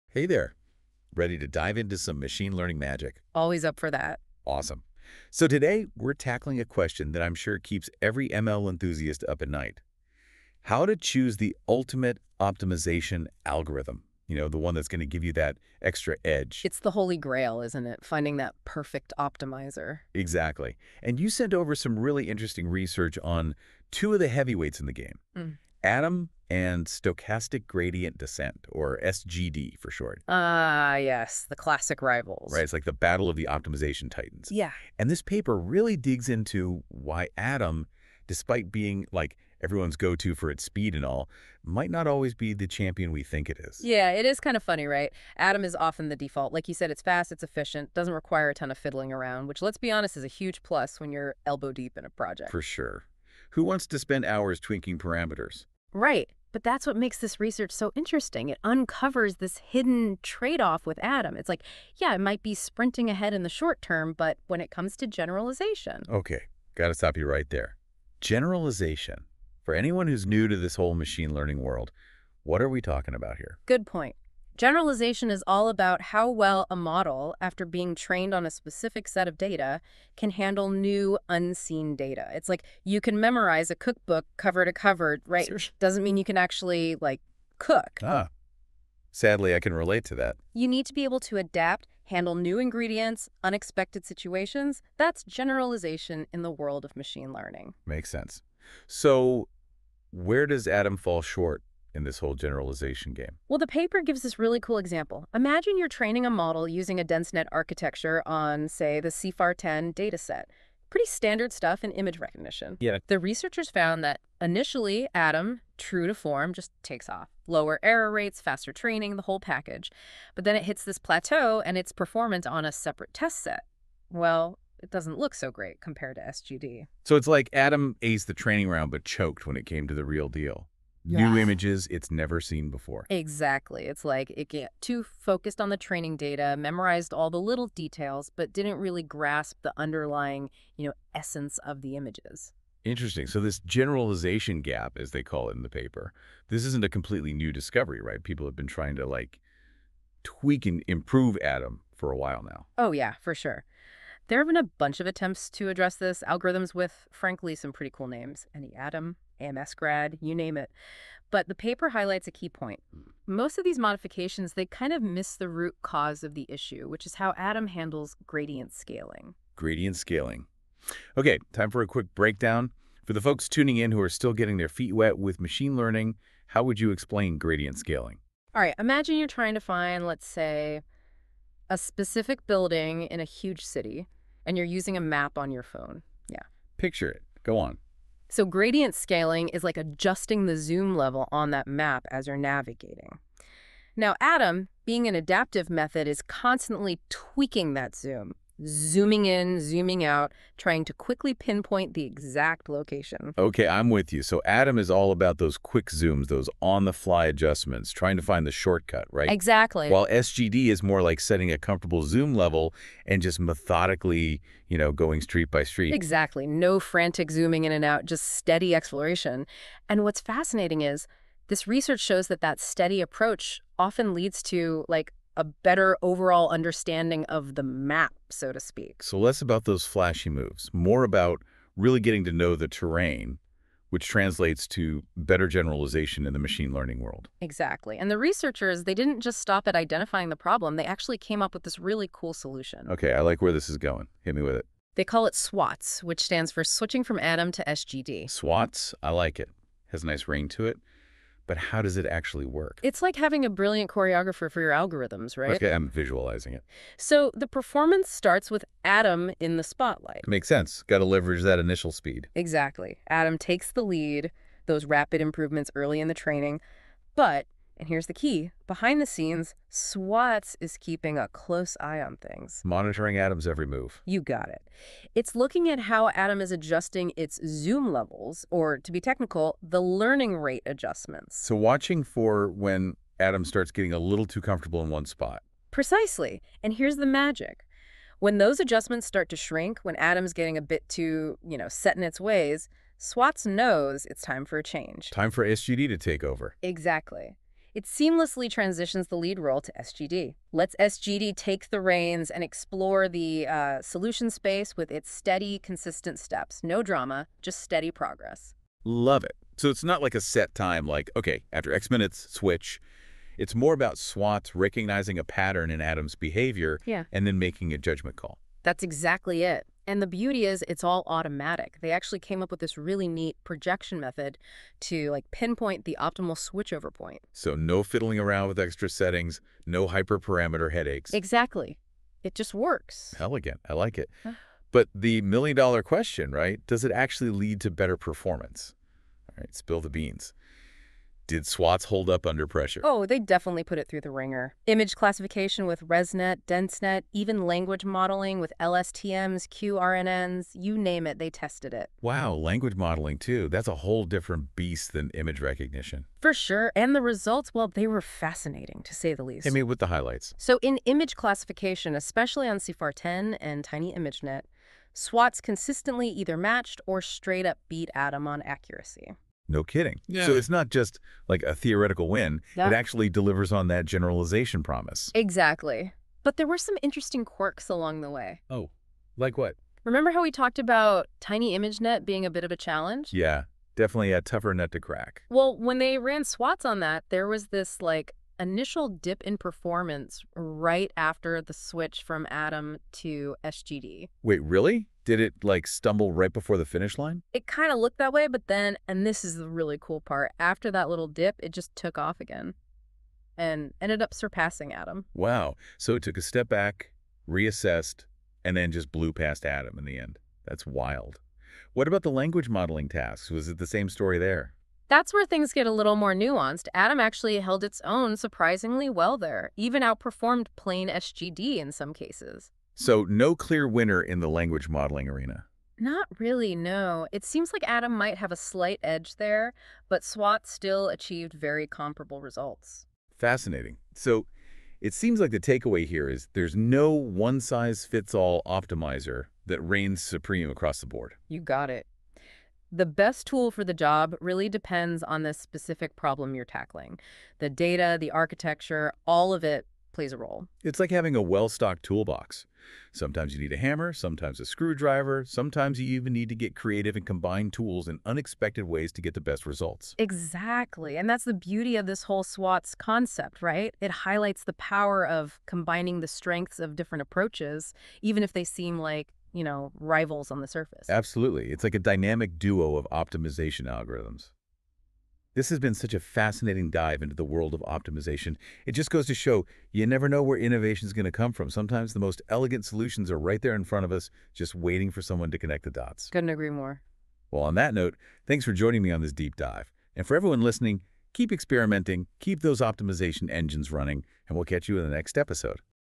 Improving Generalization Performance by Switching from Adam to SGD
This paper investigates a method called SWATS, which switches from an adaptive optimization method like Adam to SGD (Stochastic Gradient Descent) during training. This approach aims to combine the benefits of both optimizers, namely, Adam's fast initial progress and SGD's superior generalization performance. The paper proposes a criterion for determining the optimal switch point and uses a projection-based method to estimate the learning rate for SGD after the switch. The effectiveness of SWATS is demonstrated through experiments on various image classification and language modeling tasks, showing comparable performance to the best individual optimizer.